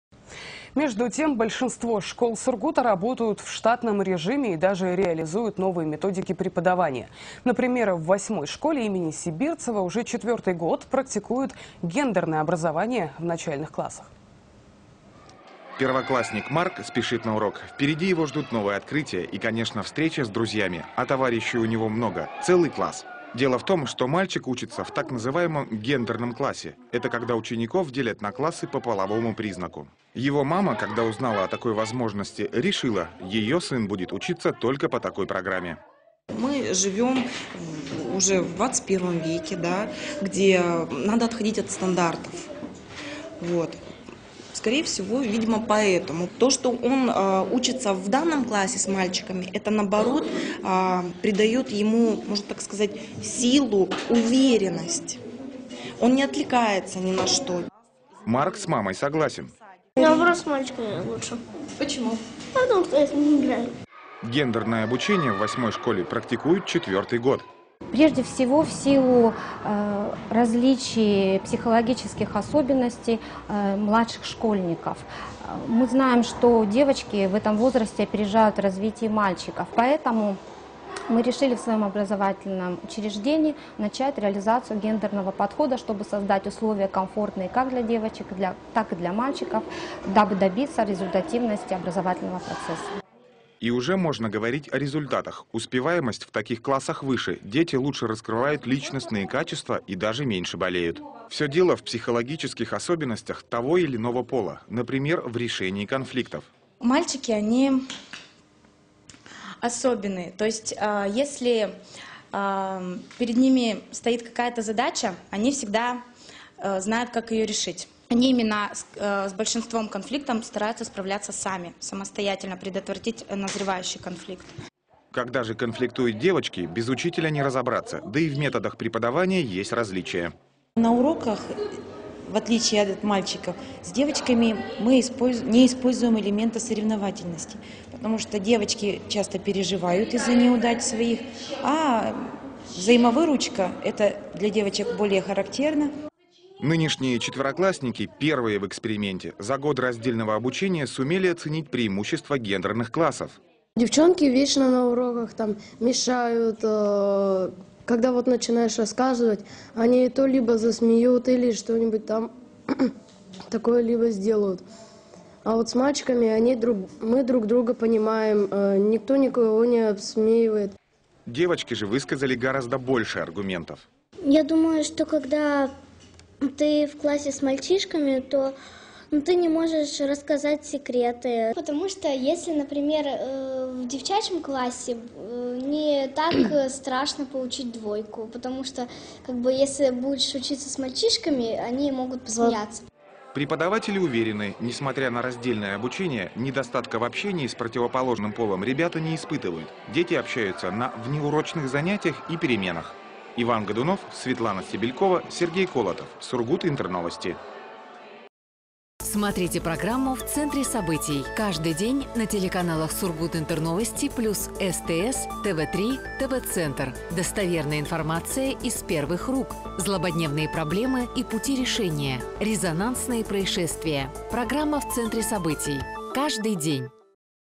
Гендерные классы (ВЦС от 25.09.14)
Большинство школ Сургута работают в штатном режиме и даже реализуют новые методики преподавания.